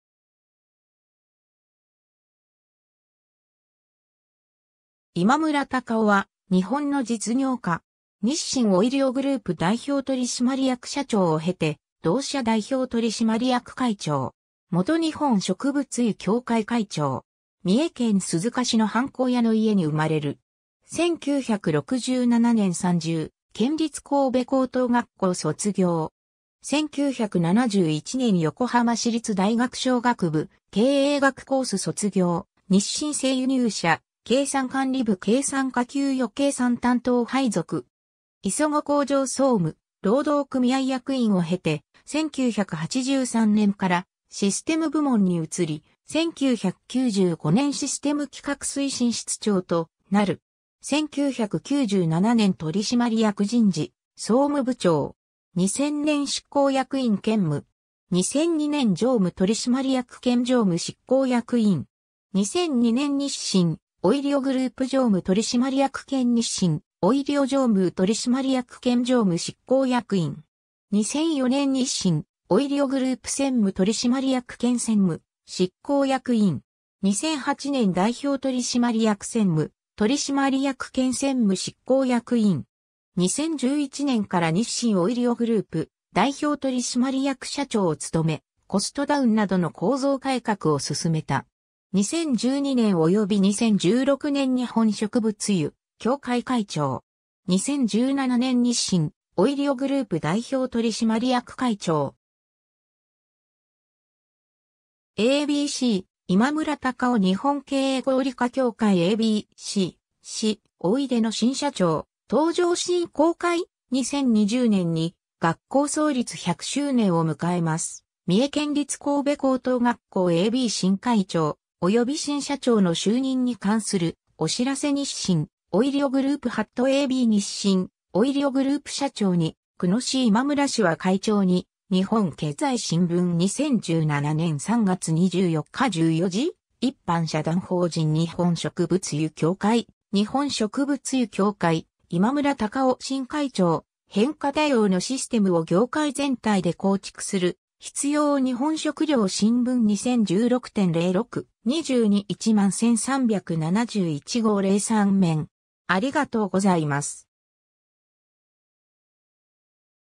今村隆郎
今村 隆郎（いまむら たかお、1949年1月8日 - ）は日本の実業家。
日清オイリオグループ代表取締役社長を経て、同社代表取締役会長。
元日本植物油協会会長。
 三重県鈴鹿市のはんこ屋の家に生まれる。
1967年三重県立神戸高等学校卒業。
1971年横浜市立大学商学部経営学コース卒業、日清製油入社、計算管理部計算課給与計算担当配属。
磯子工場総務、労働組合役員を経て、1983年からシステム部門に移り、1995年システム企画推進室長となる。
 1997年取締役人事・総務部長。
2000年執行役員兼務。
2002年常務取締役兼常務執行役員。
2002年日清オイリオグループ常務取締役兼日清オイリオ常務取締役兼常務執行役員。
2004年日清オイリオグループ専務取締役兼専務執行役員。
2008年代表取締役専務取締役兼専務執行役員。
 2011年から日清オイリオグループ代表取締役社長を務め、コストダウンなどの構造改革を進めた。
2012年及び2016年日本植物油協会会長。
2017年日清オイリオグループ代表取締役会長。
 ^ a b c 「今村隆郎（いまむらたかお）」日本経営合理化協会 ^ a b c 「市大出の新社長、登場」進交会 ^ 「2020(平成32)年に学校創立100周年を迎えます」三重県立神戸高等学校 ^ a b 新会長および新社長の就任に関するお知らせ 日清オイリオグループ ^ a b 日清オイリオグループ社長に久野氏　今村氏は会長に日本経済新聞2017/3/24 14:00 ^ 一般社団法人日本植物油協会 ^ 「日本植物油協会・今村隆郎新会長「変化対応のシステムを」　業界全体で構築する必要」日本食糧新聞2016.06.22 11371号 03面